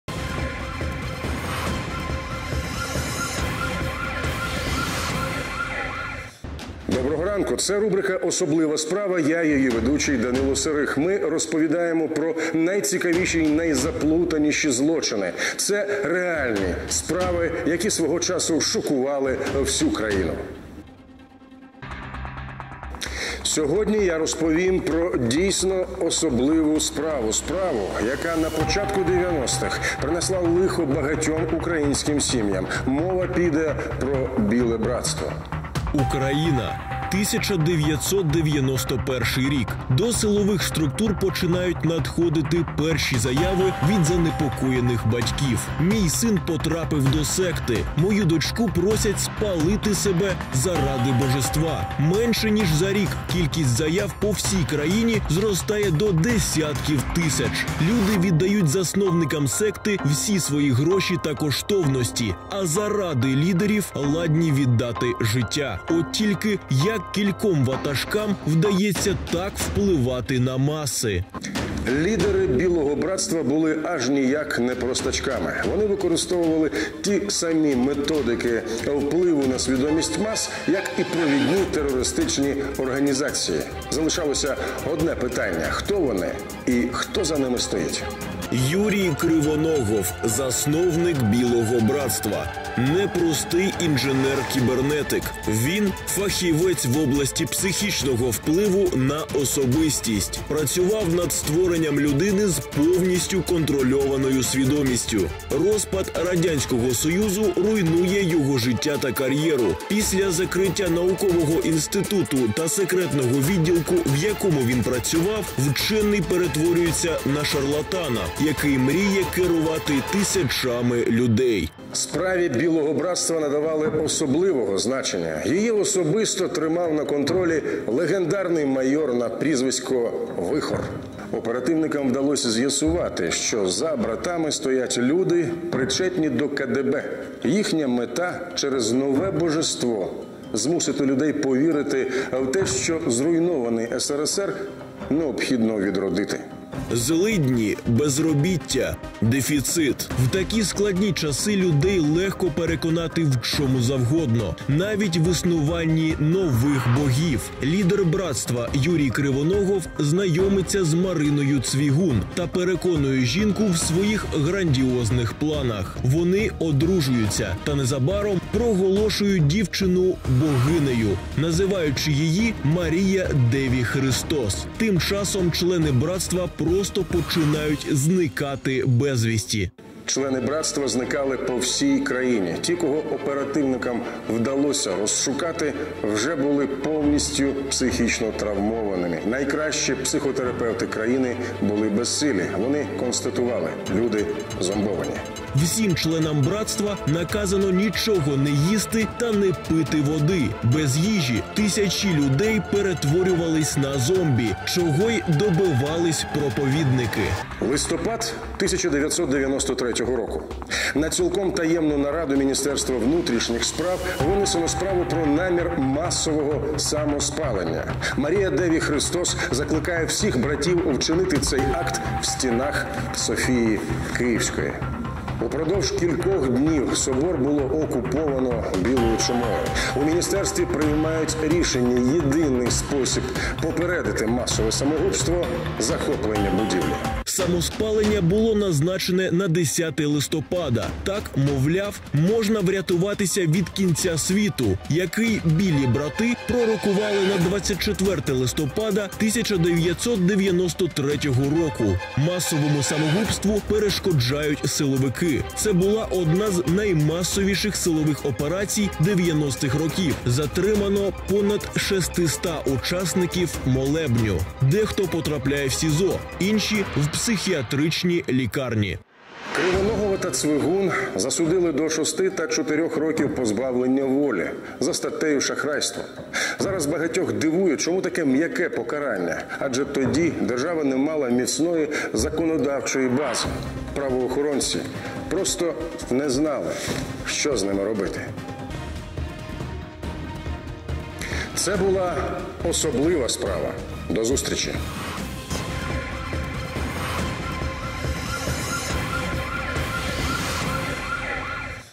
Одиозная секта «Белое братство» была проектом КГБ
В начале девяностых действовало Белое братство. Оперативникам удалось установить, что за братьями стоят люди, причастные к КГБ.